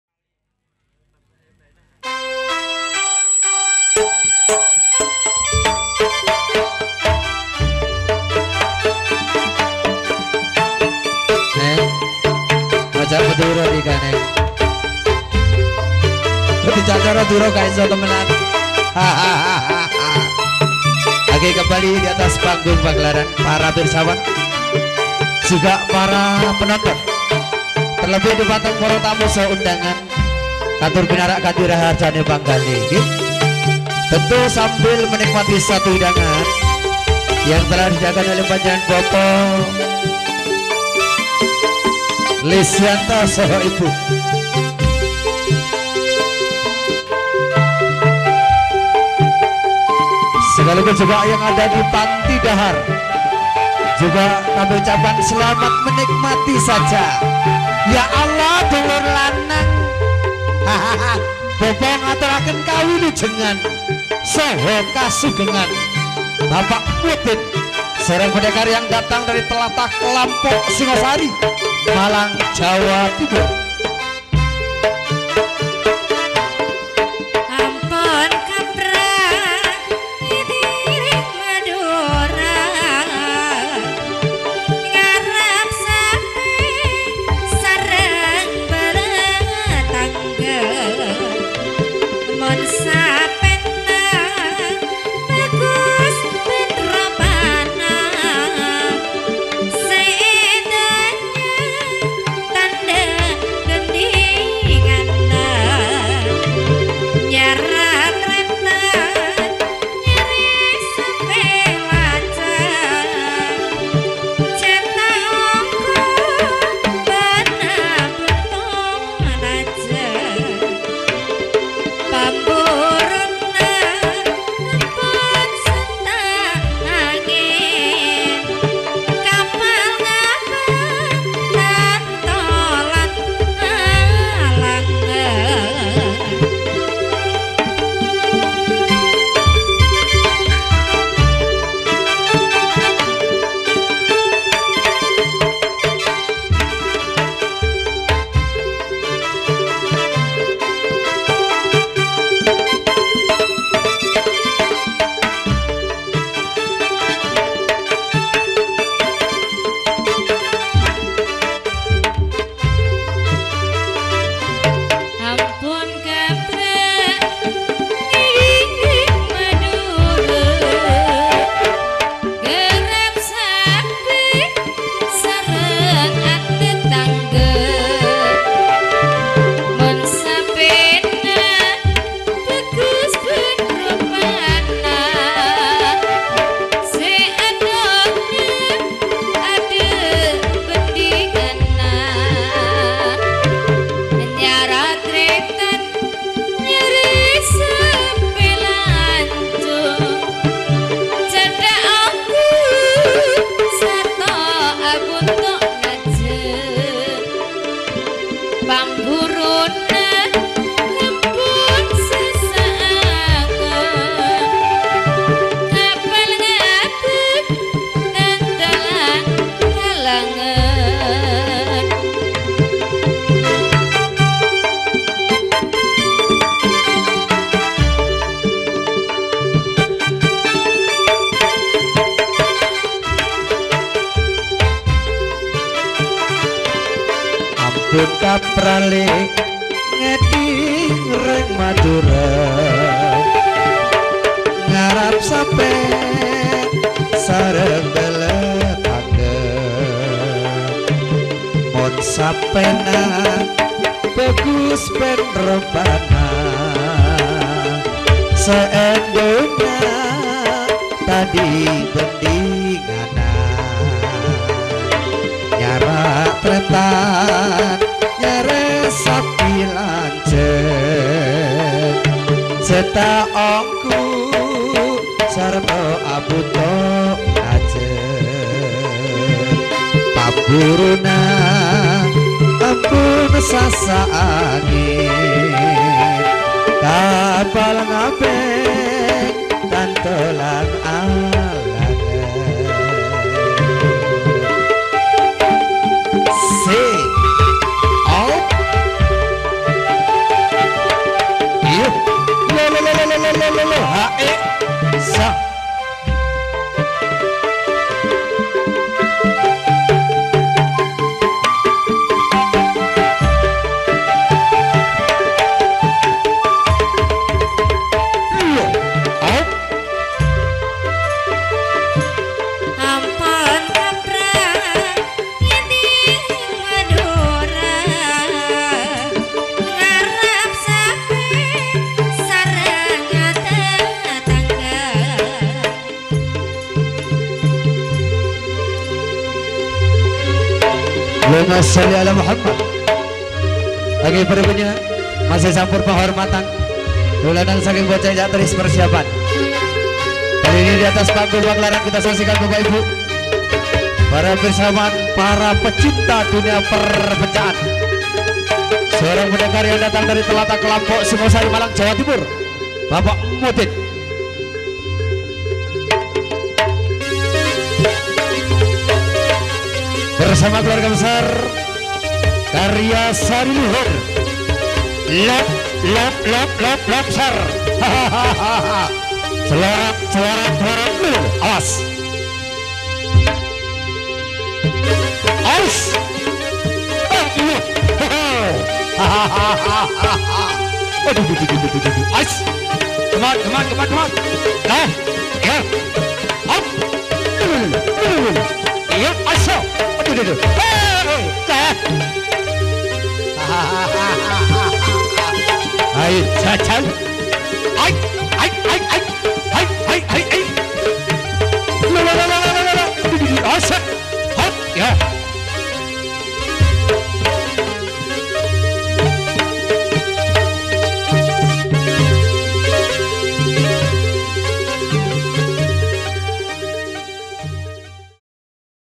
PENCAK SILAT KARYA SARI LUHUR TERBARU KEMBANGAN BPK. MODIN AZIZ
Atraksi ini adalah salah satu atraksi dalam pencak silat, bersama persatuan pencak silat karya sari luhur batu  malang, para pesilat ini dengan sangat bagus membawakanya.